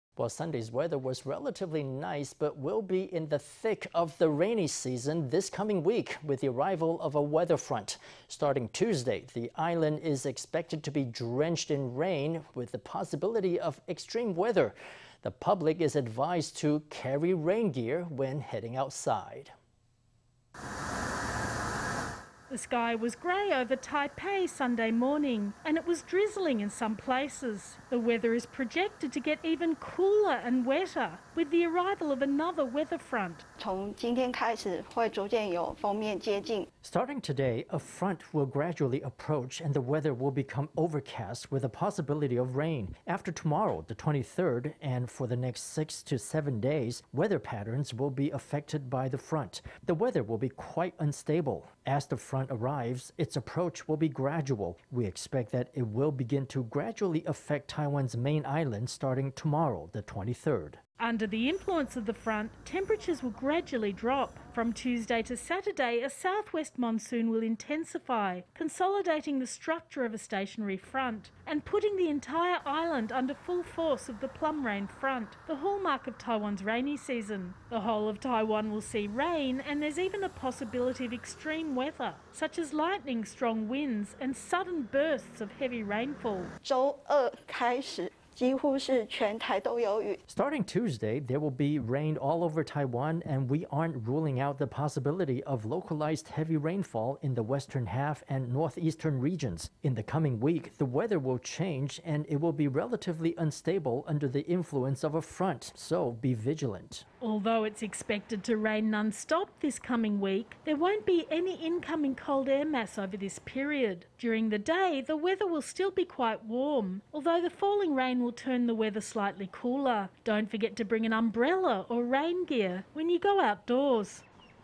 Plum rain front, hallmark of Taiwan’s rainy season, to be in full force starting Tuesday
Sunday''s weather was relatively nice, but we''ll be in the thick of the rainy season this coming week with the arrival of a weather front. Starting Tuesday, the island is expected to be drenched in rain, with the possibility of extreme weather. The public is advised to carry rain gear when heading outside.

The sky was gray over Taipei Sunday morning and it was drizzling in some places. The weather is projected to get even cooler and wetter with the arrival of another weather front.

Chen Yi-hsou
Forecaster, Central Weather Bureau
Starting today, a front will gradually approach and the weather will become overcast with a possibility of rain. After tomorrow, the 23rd, and for the next six to seven days, weather patterns will be affected by the front. The weather will be quite unstable. As the front arrives, its approach will be gradual. We expect that it will begin to gradually affect Taiwan’s main island starting tomorrow, the 23rd.

Under the influence of the front, temperatures will gradually drop. From Tuesday to Saturday, a southwest monsoon will intensify, consolidating the structure of a stationary front and putting the entire island under full force of the plum rain front, the hallmark of Taiwan’s rainy season. The whole of Taiwan will see rain, and there’s even a possibility of extreme weather, such as lightning, strong winds and sudden bursts of heavy rainfall.

Chen Yi-hsou
Starting Tuesday, there will be rain all over Taiwan and we aren’t ruling out the possibility of localized heavy rainfall in the western half and northeastern regions. In the coming week, the weather will change and it will be relatively unstable under the influence of a front, so be vigilant.

Although it’s expected to rain non-stop this coming week, there won’t be any incoming cold air mass over this period. During the day, the weather will still be quite warm, although the falling rain will turn the weather slightly cooler. Don’t forget to bring an umbrella or rain gear when you go outdoors.